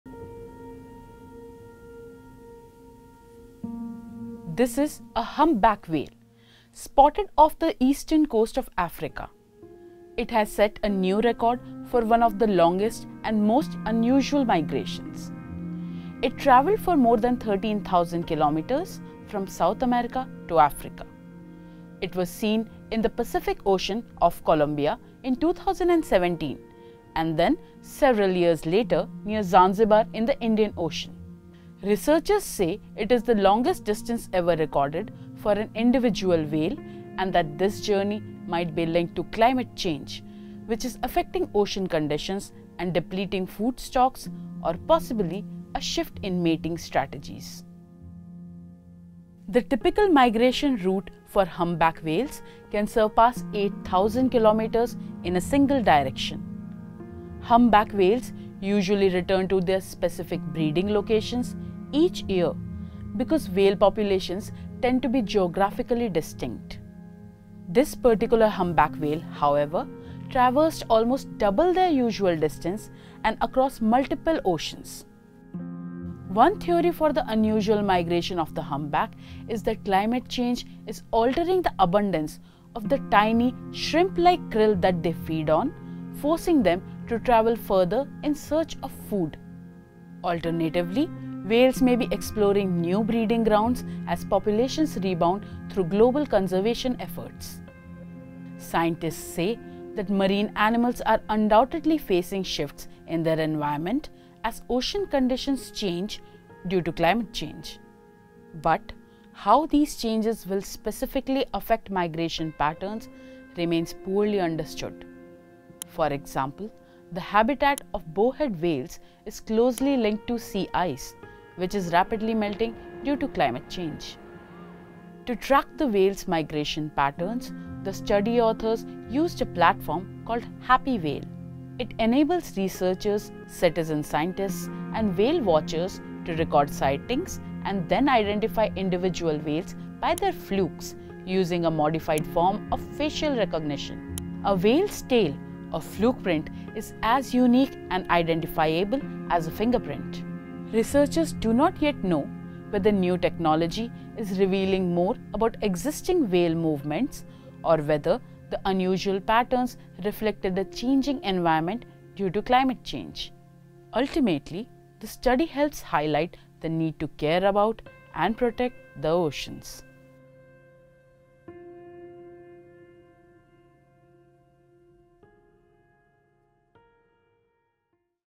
Humpback Whale Completes Epic 13,000 km Migration Across Oceans, Driven by Climate Change?
A humpback whale has stunned scientists by completing one of the longest migrations ever recorded. Starting off the coast of Colombia in 2017, the whale traveled over 13,000 km to appear near Zanzibar in the Indian Ocean years later. Experts believe climate change may have disrupted food sources, or the whale may have been on a quest to find a mate. This extraordinary journey is being hailed as an "impressive and unusual" feat for the highly migratory species, according to experts. Watch the video to know more.